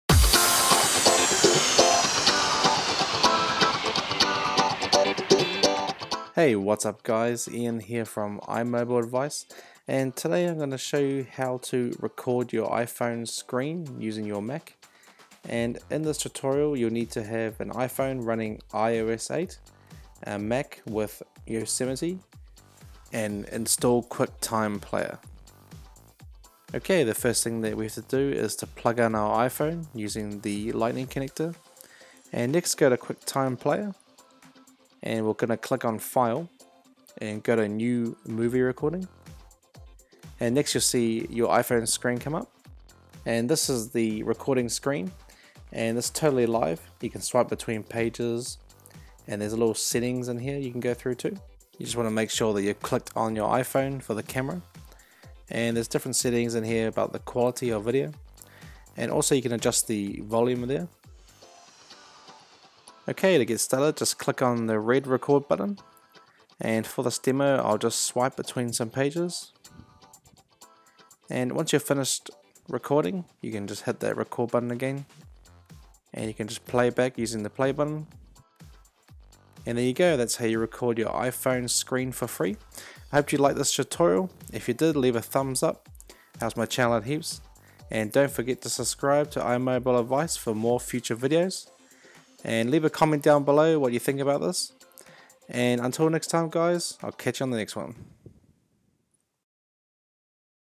How to record your iPhone screen for FREE
Hey everyone. In this video tutorial I will show you how to record your iPhone screen on your Mac. This is very simple to do and best of all its FREE.